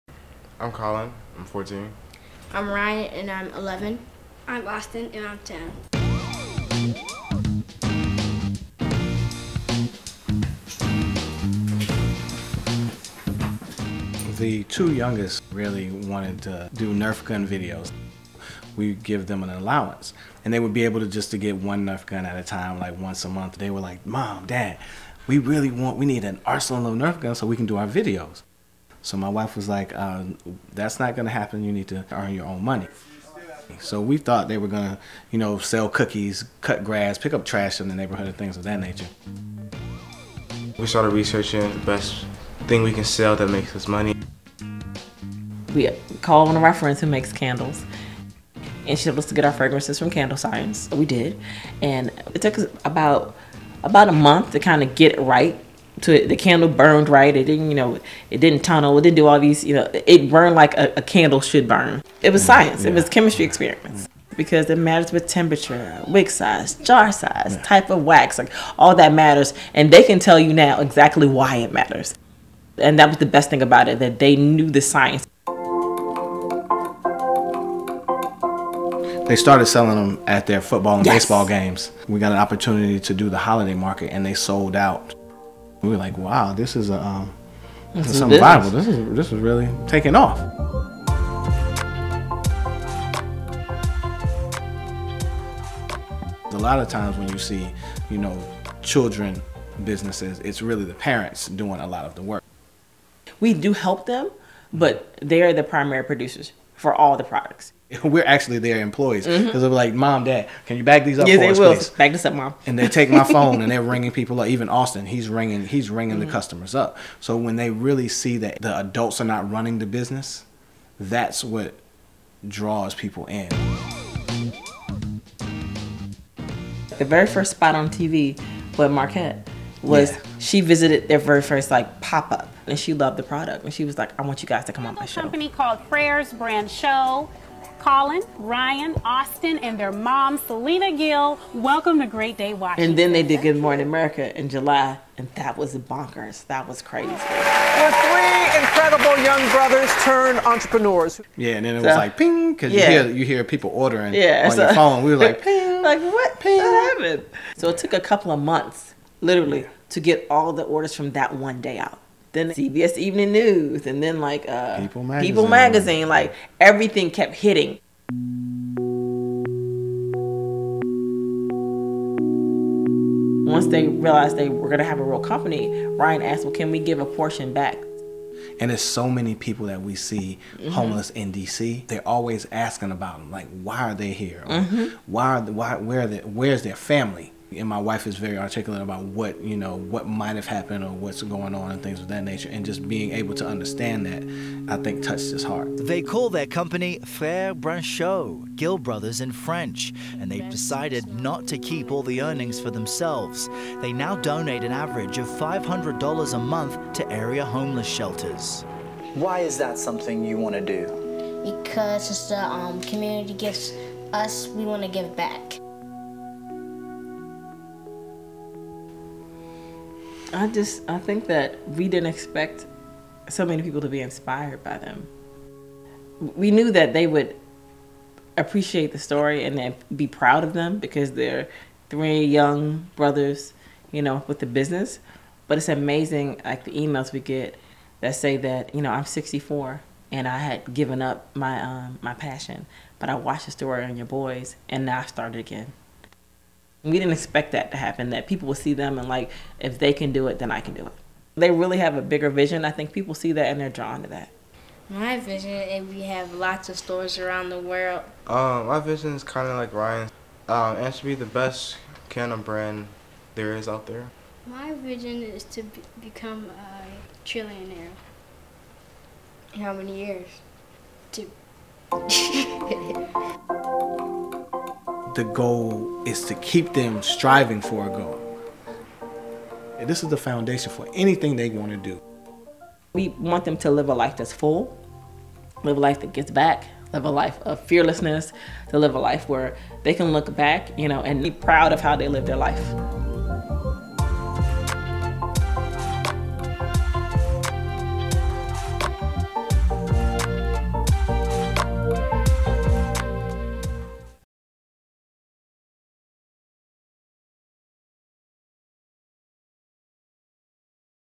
Life Lessons Through Candle Making — Talking with Frères Branchiaux Candle Co. | CandleScience
What began as a way to earn extra spending money, give back to the community, and learn about running a business is now a thriving candle brand. Brothers Collin, Austin, and Ryan Gill are the dedicated young entrepreneurs behind Frères Branchiaux Candle Company. We had a blast learning more about this amazing family and are honored to share their story with you.